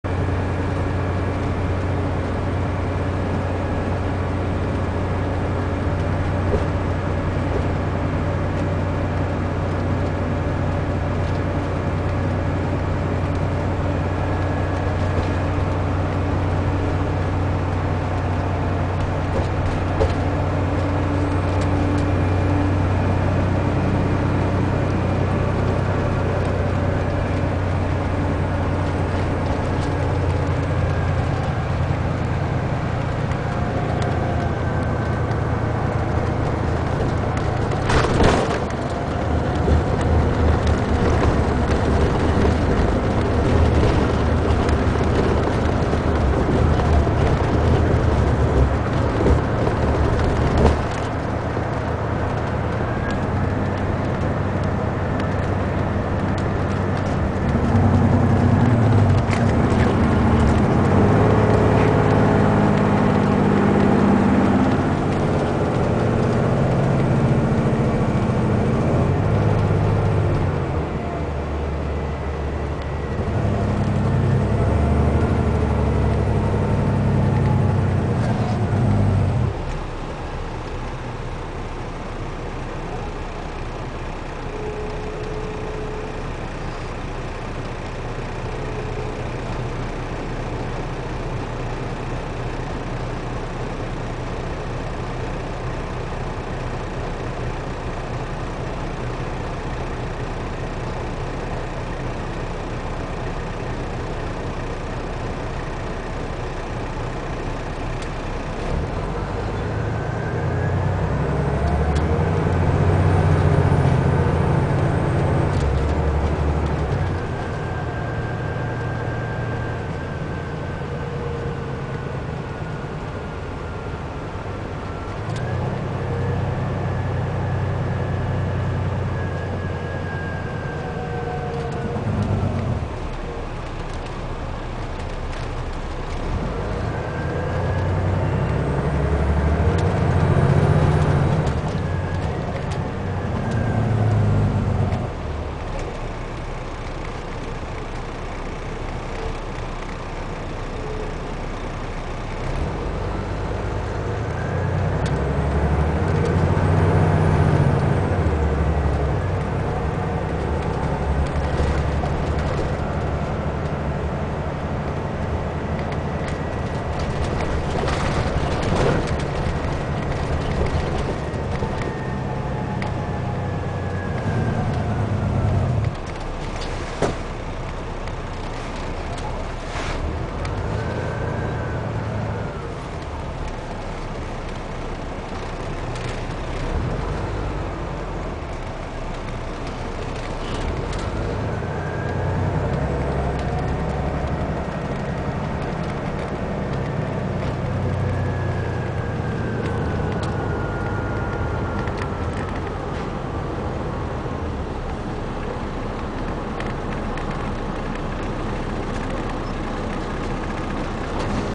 New Jersey Transit D4000 #7986
on the Atlantic City Expressway on a 319 run. The 319 is suppose to be a D4500 w/ a Restroom but since 7183 broke down in NY before the run, the Driver went and got a D4000 from the Medowlands.

7986 is a Medowlands Bus on a Egg Harbor Route.

These buses have DD 60 Series Engine Allsion B500 WTEC Transmission w/ Jake Brake.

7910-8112 has the loudest Jake Brakes.